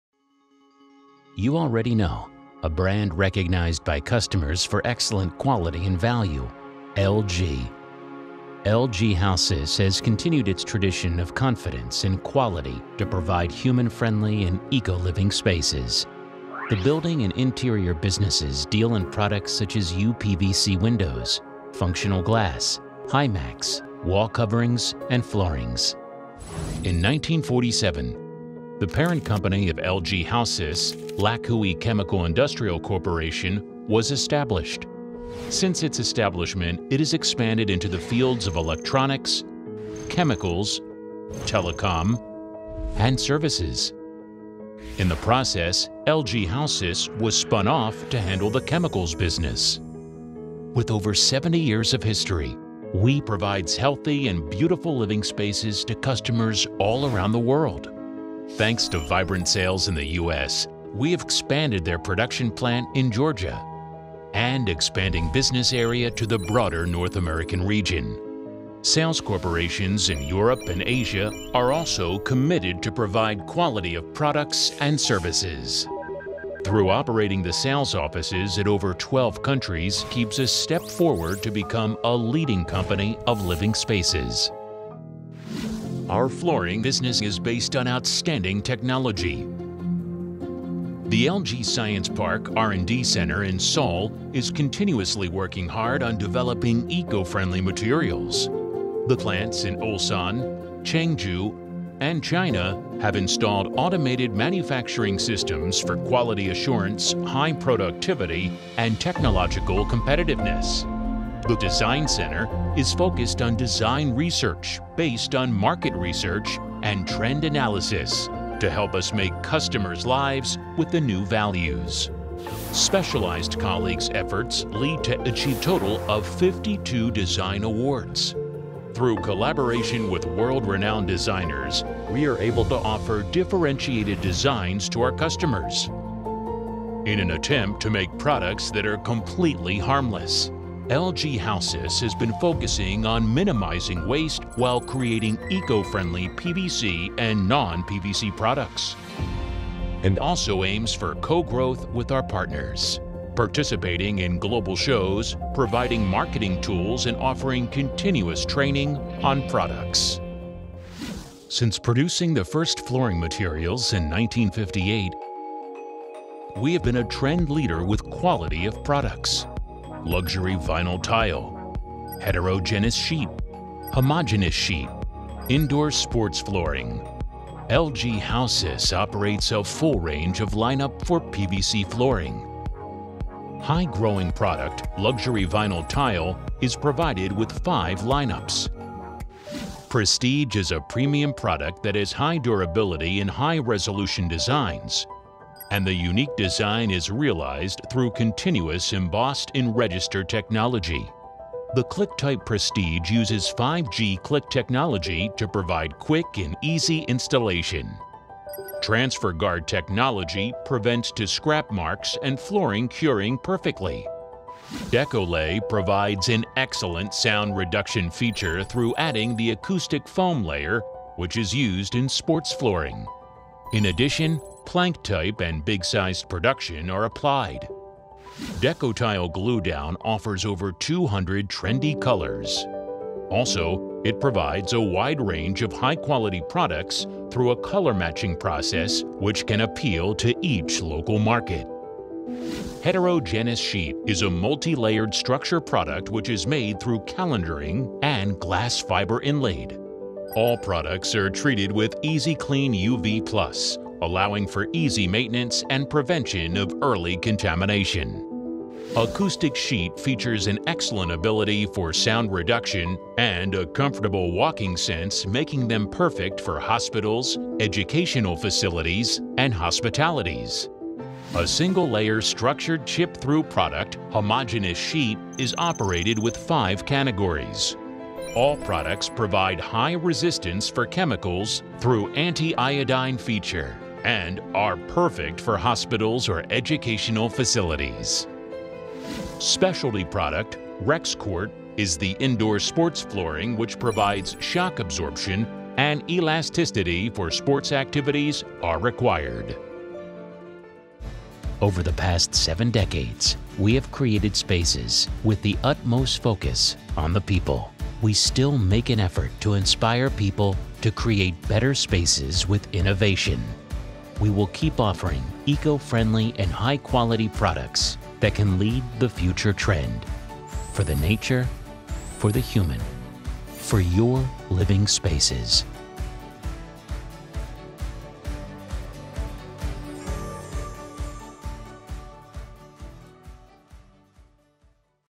LG Hausys Flooring Introduction(TW)
You can see who we are, what we produce. We produce Flooring product such as LVT, Heterogeneous sheet and Homogeneous sheet.